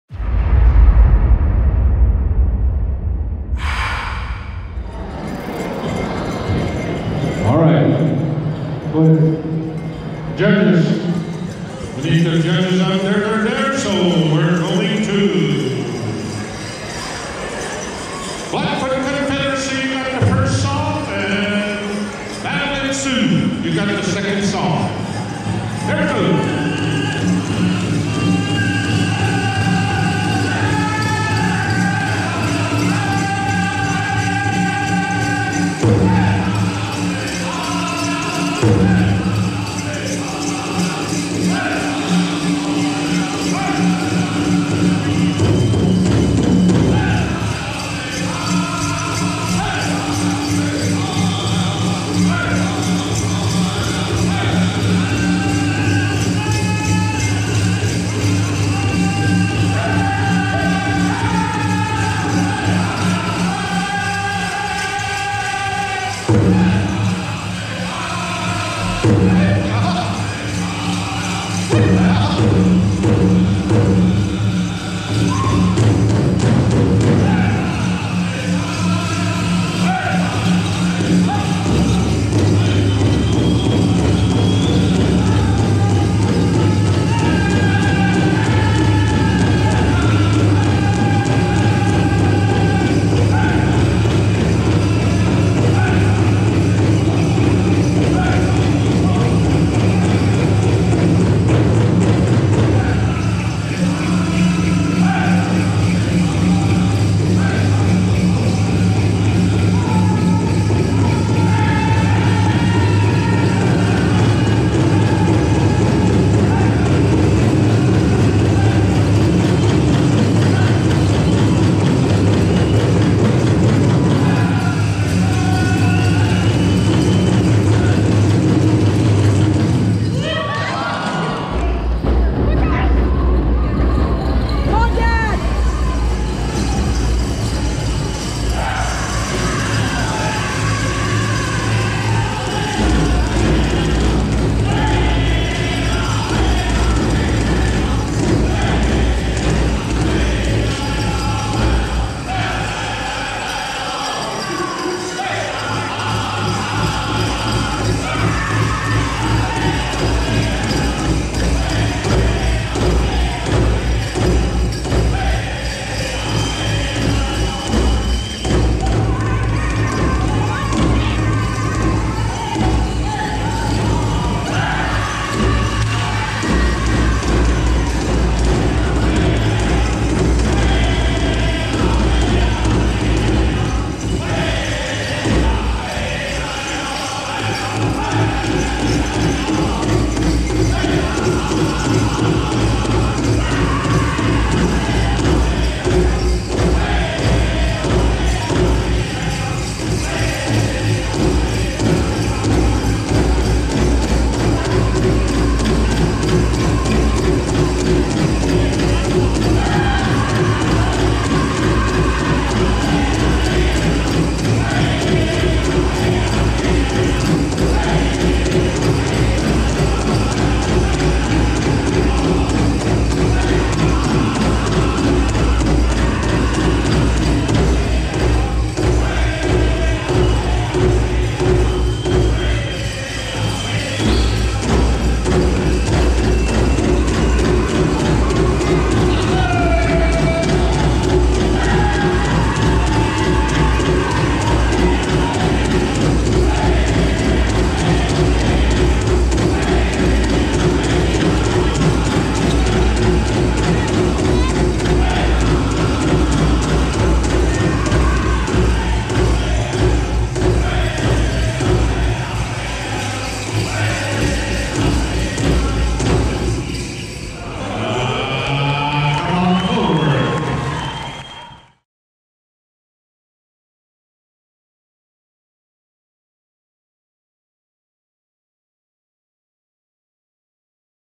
Jr Mens Fancy 2 Songs | Powwow Times
Jr Mens Fancy 2 Songs @ Siksika Powwow 2019